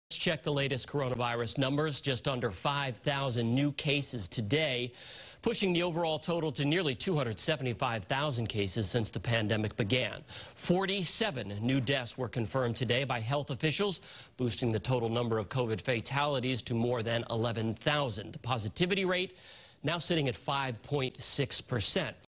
Massachusetts Reports 4,968 New COVID Cases, 47 Additional Deaths
Health officials said the total number of confirmed cases in the state is now 274,897 while the total number of deaths is 11,057.